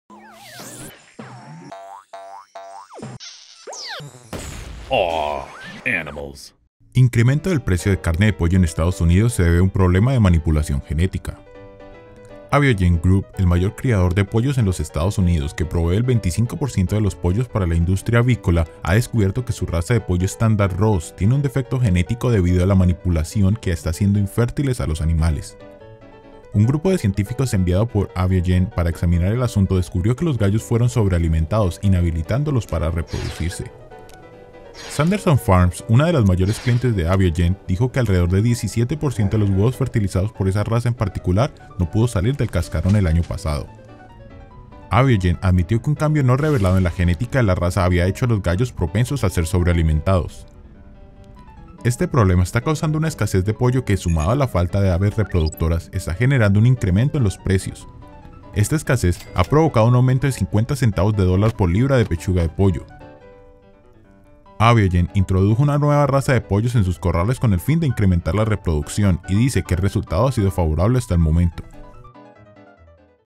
Incremento del precio de carne de pollo en EE.UU se debe a un problema de manipulación genética
Aviagen Group, el mayor criador de pollos en los EE.UU., que provee el 25% de los pollos para la industria avícola en los Estados Unidos ha descubierto que su raza de pollo estándar "Ross" tiene un defecto genético debido a la manipulación genética que está haciendo infértiles a los animales.

Un grupo de científicos enviado por Aviagen para examinar el asunto descubrió que los gallos fueron sobrealimentados, inhabilitándolos para reproducirse.

Sanderson Farms, uno de los mayores clientes de Aviagen dijo que alrededor del 17% de los huevos fertilizados por esa raza en particular no pudo salir del cascarón el año pasado.

Aviagen admitió que un cambio no revelado en la genética de la raza había hecho a los gallos sensibles a ser sobrealimentados.

Este problema está causando una escasez de pollo que sumado a la falta de aves reproductoras, están generando un incremento en los precios del pollo. Esta escasez ha provocado un aumento de 50 centavos de dólar por libra de pechugas de pollo.

Aviagen introdujo una nueva raza de pollos en sus corrales con el fin de incrementar la reproducción de los animales, y dice que el resultado ha sido favorable hasta el momento.